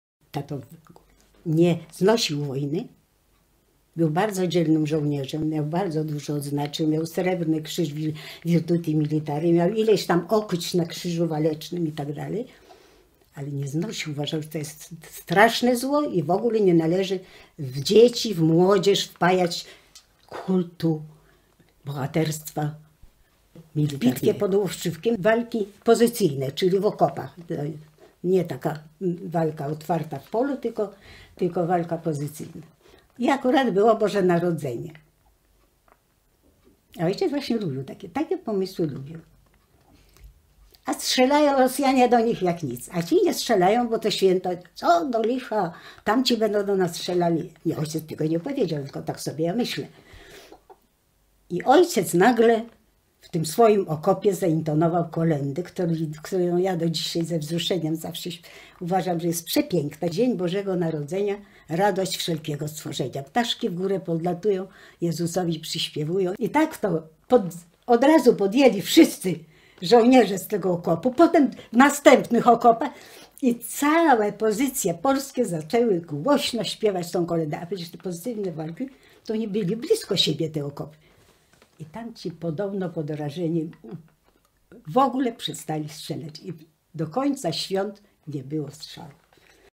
Krystyna Małkowska o śpiewaniu kolęd w okopach
Wypowiedź pani Krystyny Małkowskiej, córki legionisty Władysława Malskiego.